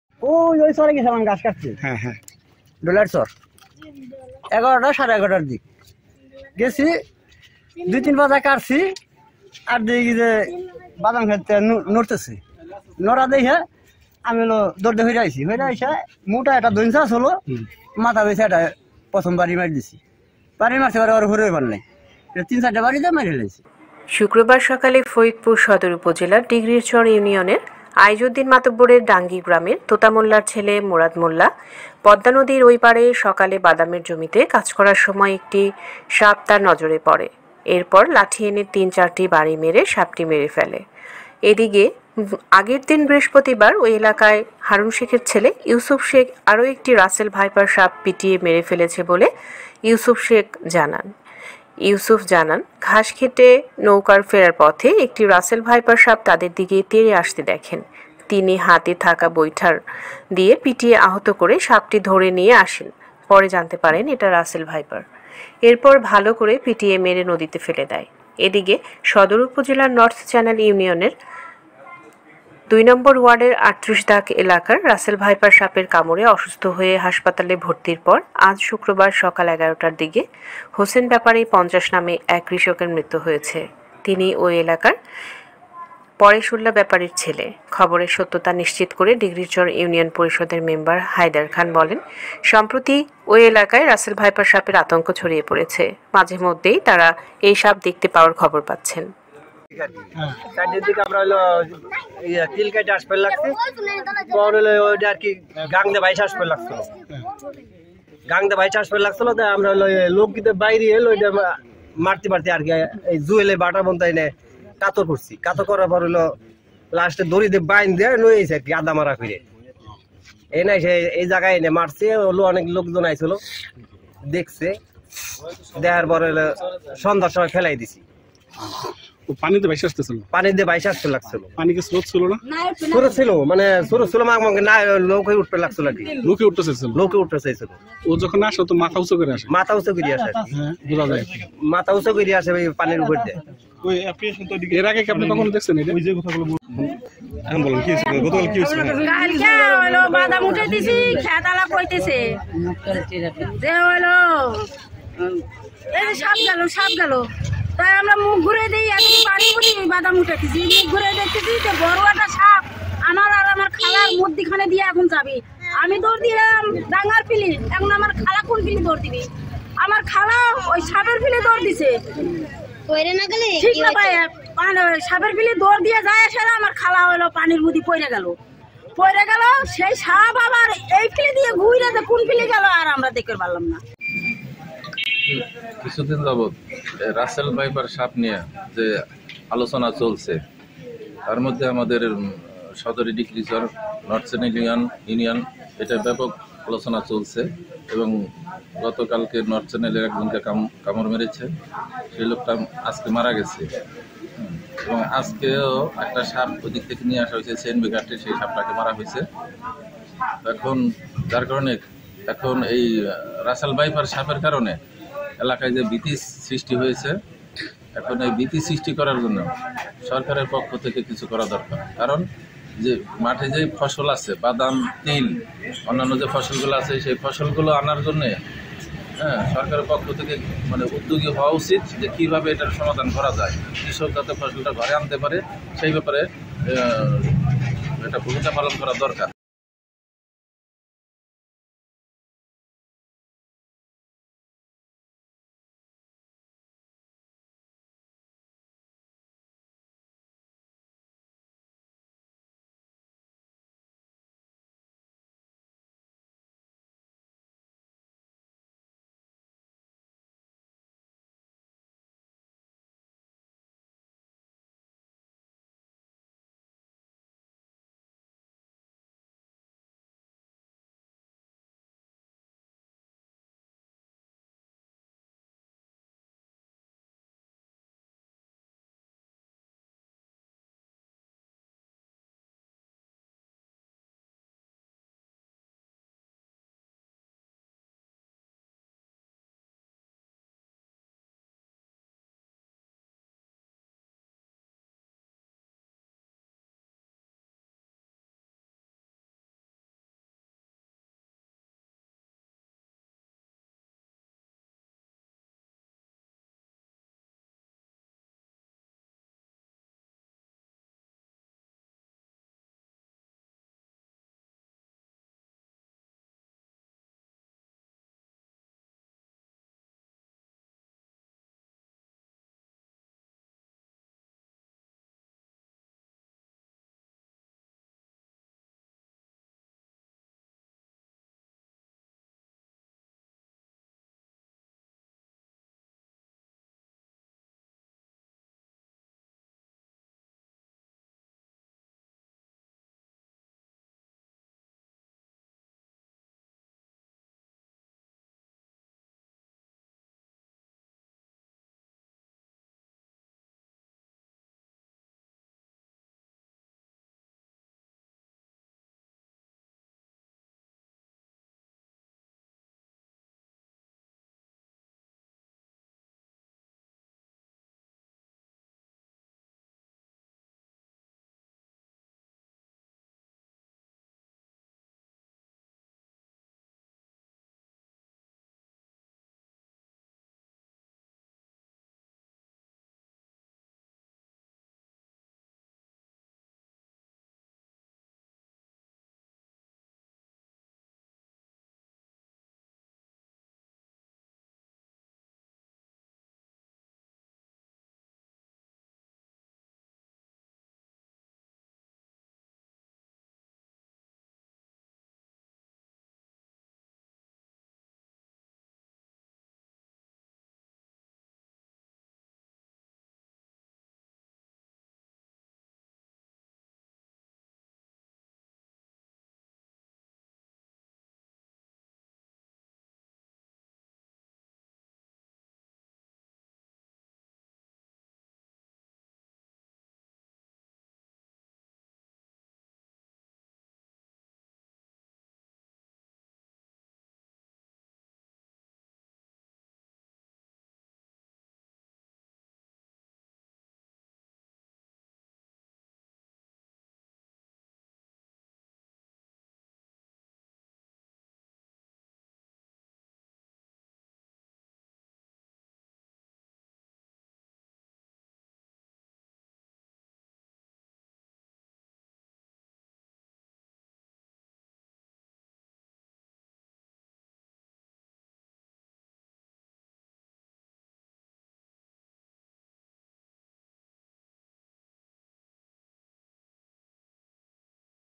পদ্মার চরে দুটি রাসেল ভাইপার মে*রে দিলো দুই বাহাদুর কৃষক, কামুড়ে একজনের মৃ*ত
ফরিদপুর সদর উপজেলার ডিক্রিরচর ইউনিয়নের আইজউদ্দীন মাতুব্বরের ডাঙ্গি  গ্রামের দুই কৃষক দুই দিনে দুটি রাসেল ভাইপার সাপ মে*রে বাহাদুর। নর্থচ্যানেলে কামুড়ে একজনের মৃঁতু।